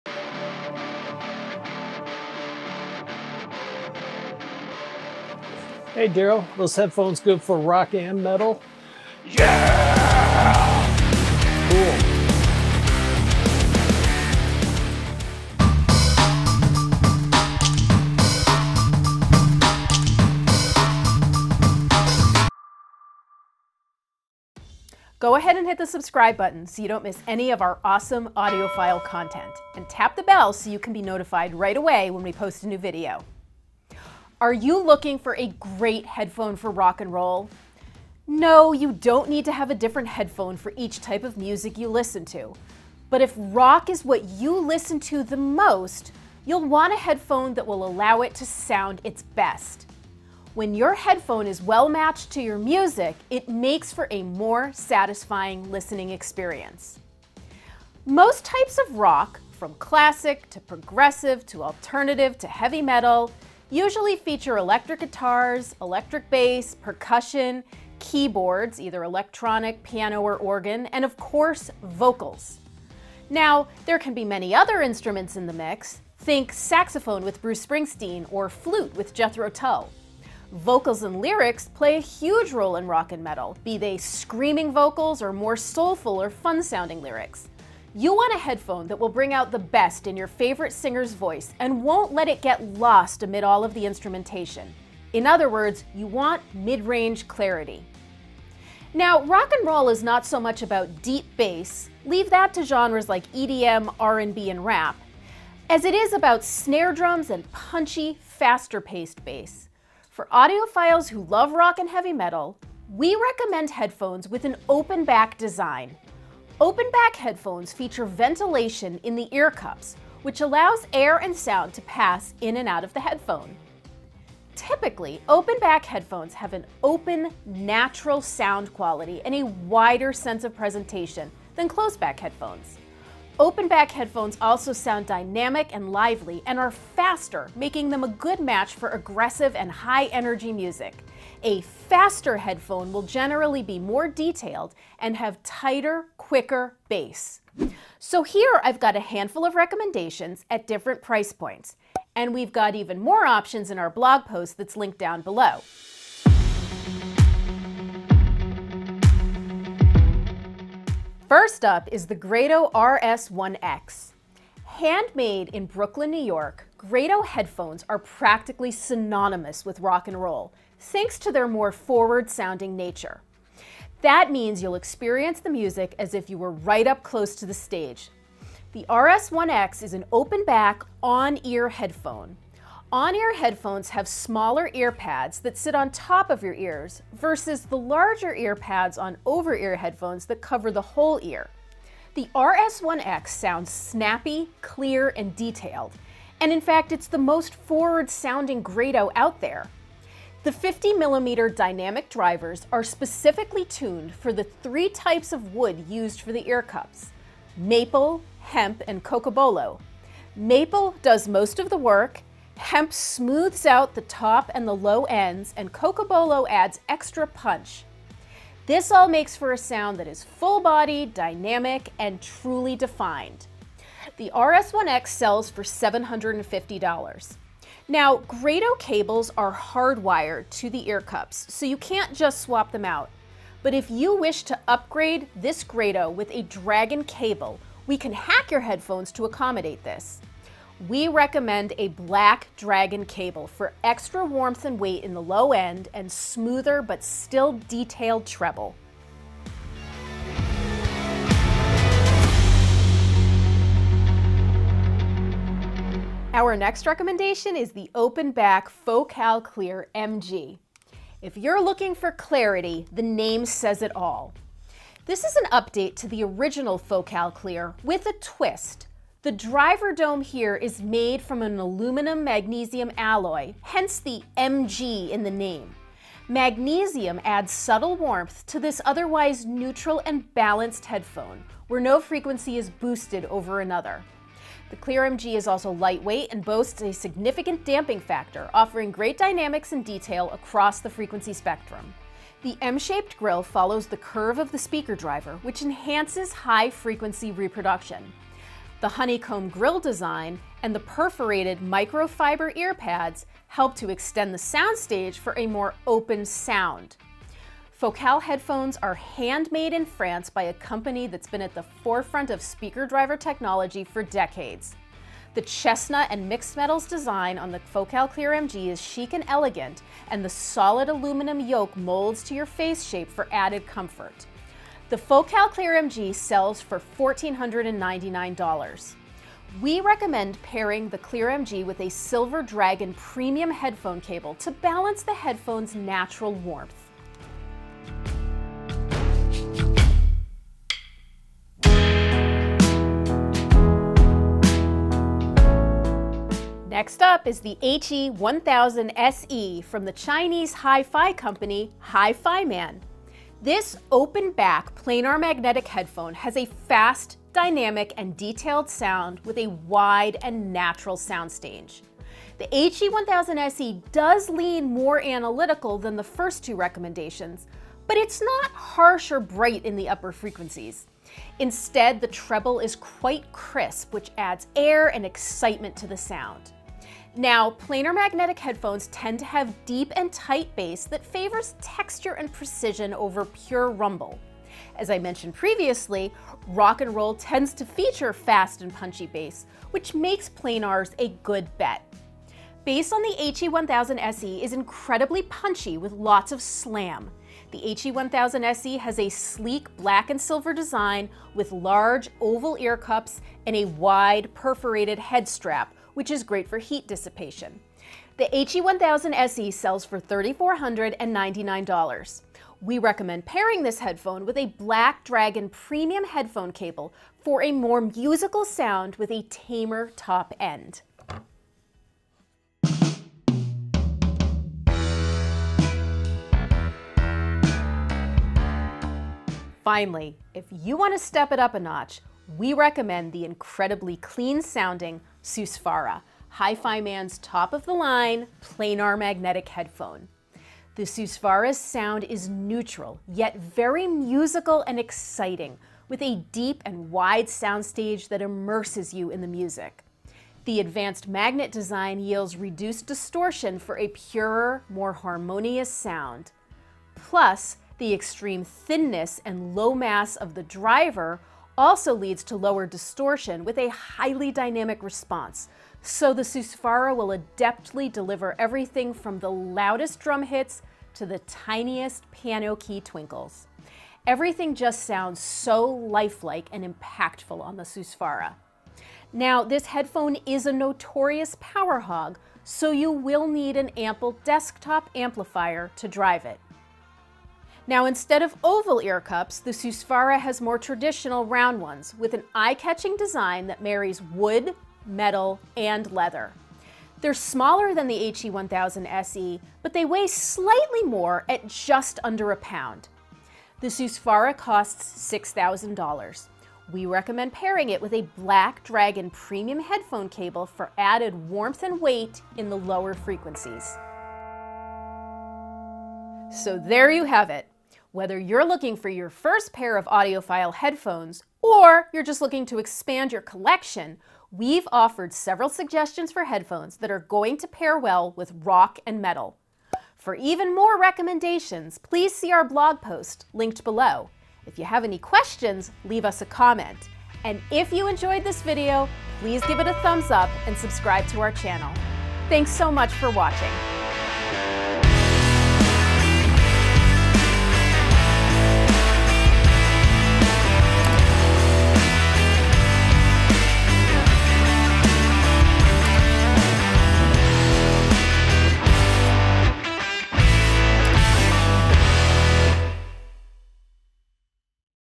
Best Audiophile Headphones for Rock, Metal Music | Moon Audio
We've curated a list of what we consider to be our best headphones for rock music and heavy metal. These genres usually feature electric guitars, electric bass, percussion, keyboards (electronic, piano, or organ), and, of course, vocals. There can be many other instruments in the mix: think sax with Bruce Springsteen, flutes in Jethro Tull, or bagpipes in Korn. Vocals and lyrics also play a huge role in rock and metal, be they screaming vocals or more soulful or fun-sounding lyrics. You want a headphone that will bring out the best in your favorite singer's voice and won't let it get lost amid the instrumentation. When your headphone is well-matched to your music, it makes for a better listening experience.

Ready to become an audiophile?

*CHAPTERS*
0:00 | Yeaaah!
0:25 | Overview & Introduction
0:58 | Why a specific headphone?
1:50 | Open-back headphones
2:36 | Grado RS1x: $750
4:16 | Focal Clear Mg: $1,499
5:42 | HiFiMan HE1000se: $3,499
7:05 | HiFiMan Susvara: $6,000
8:36 | Looking for more headphones?
9:05 | Learn More